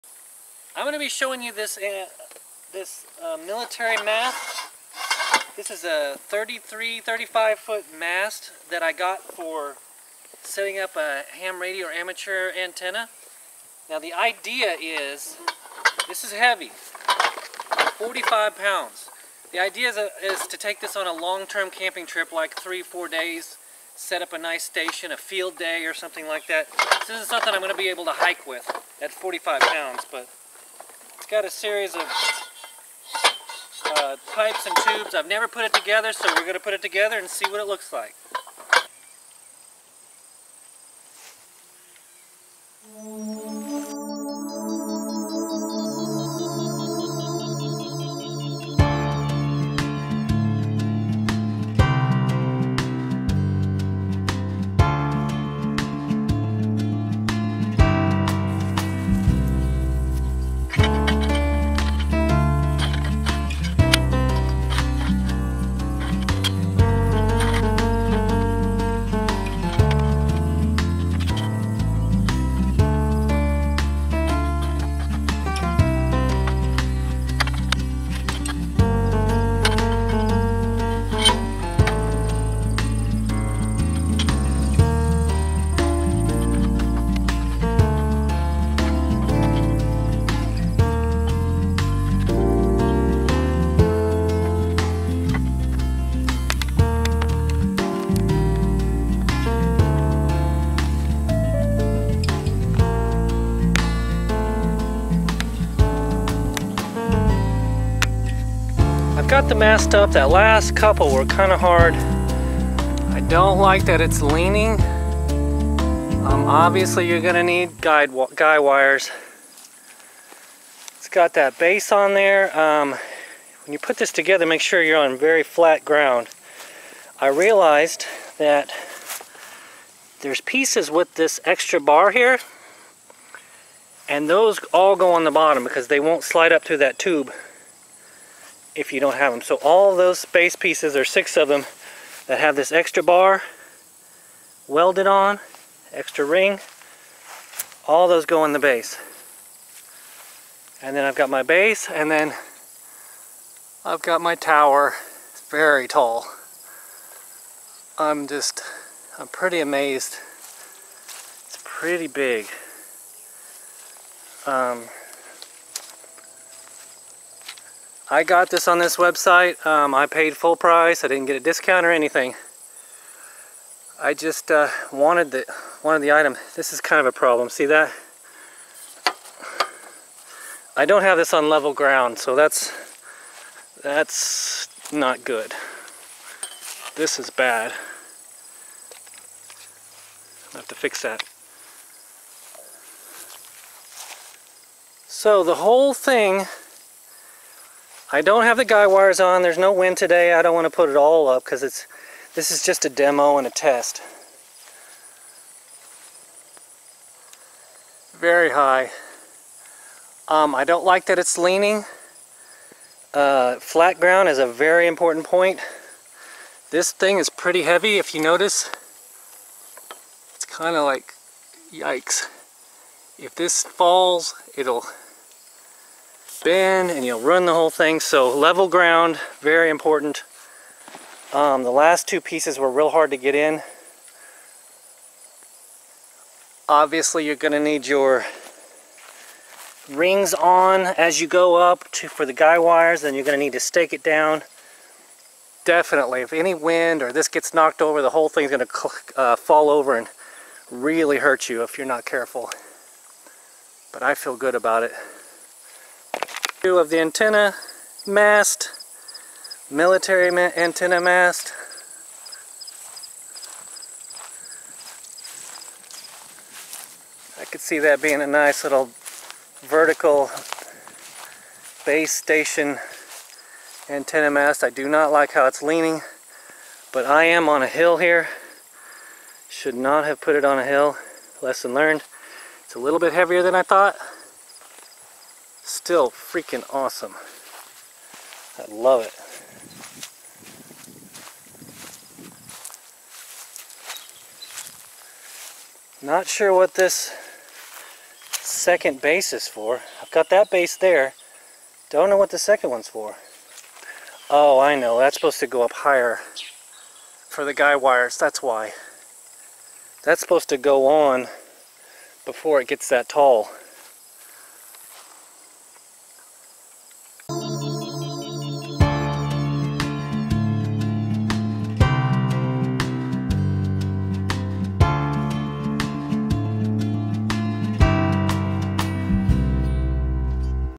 Ham Radio Antenna Military Mast Tower, Setup & Review
🔥 Jacktite Fiberglass Mast
🔥 Sling Shot 4 tree antenna

Are you looking to install a 33 foot portable military radio tower? Look no further! This step-by-step video guide will show you exactly how to do it from start to finish. With clear instructions and helpful tips, you'll be up and running in no time. Tune in now to learn everything you need to know about installing a radio tower!
00:00 Introduction
00:44 Assembly
01:27 Final Assembly
01:37 Assembled Overview
05:17 Disassemble Tower